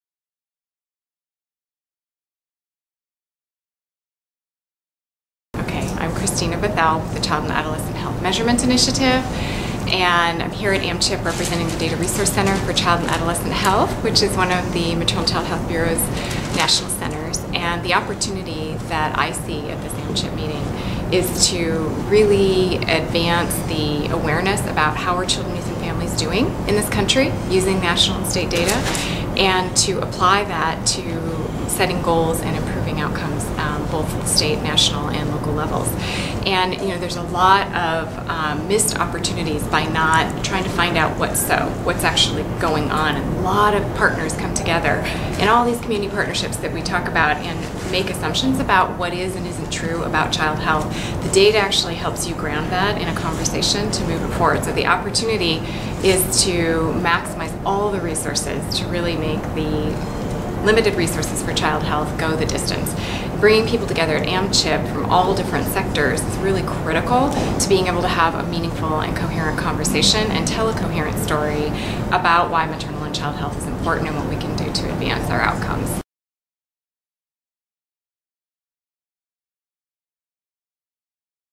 Comments from Christina Bethell, PhD, MPH, MBA, Director, CAHMI
speaks to the importance of data and collaboration in creating a coherent approach to driving more successful outcomes in maternal and child health.